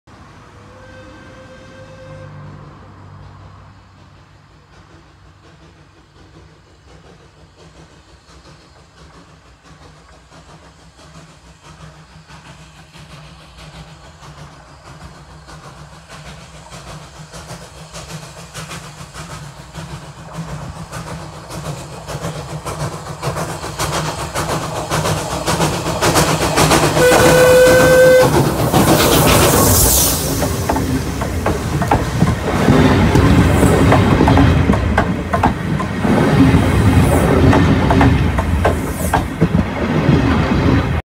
An alternate view
A short video of Bon-Accord hauling a train along the Royal Deeside Railway. This was one of the first weekends that Bon-Accord operated with full steam heat train carriage heating.